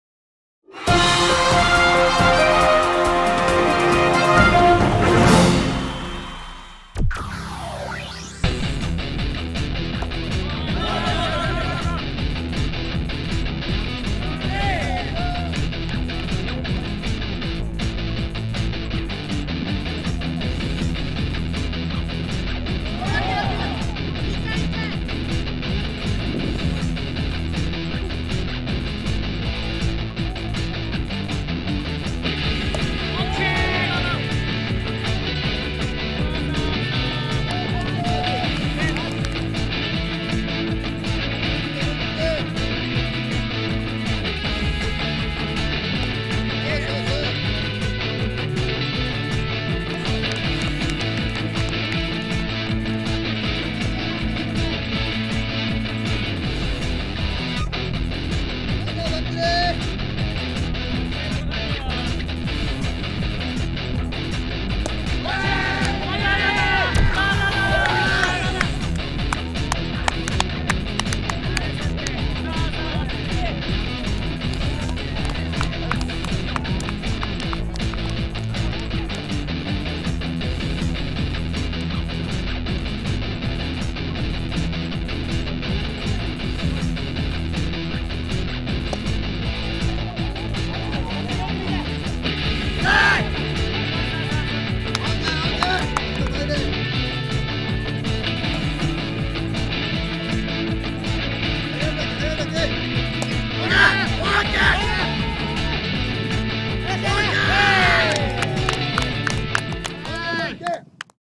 ドージーズ×オールディーズ 戦
2012年7月15日（日）、洗堰北(B)球場で行われたドージーズ×オールディーズ 戦。首位決戦の大事な試合、ドージーズは3回表に3点を先制する！その得点シーンを動画でお届け！！